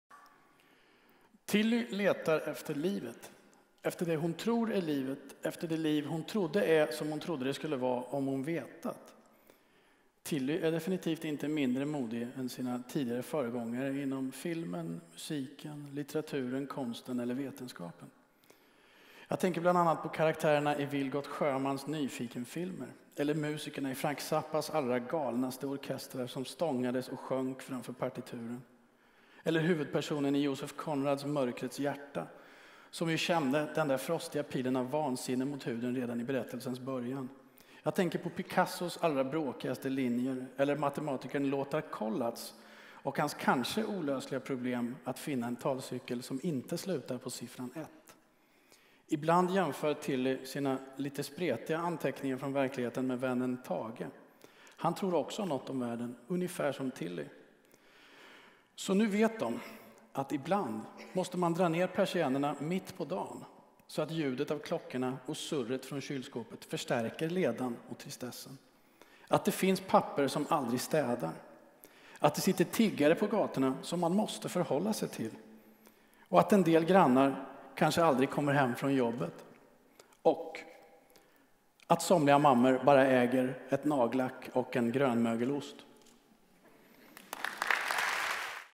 Augustgalan 2014: Ulf Stolt presenterar Tilly som trodde att... av Eva Staaf och Emma Adbåge
Tilly som trodde att... av Eva Staaf och Emma Adbåge är nominerad till Årets svenska barn- och ungdomsbok. På Augustgalan presenterades boken av Ulf Stolt, chefredaktör för Situation Stockholm.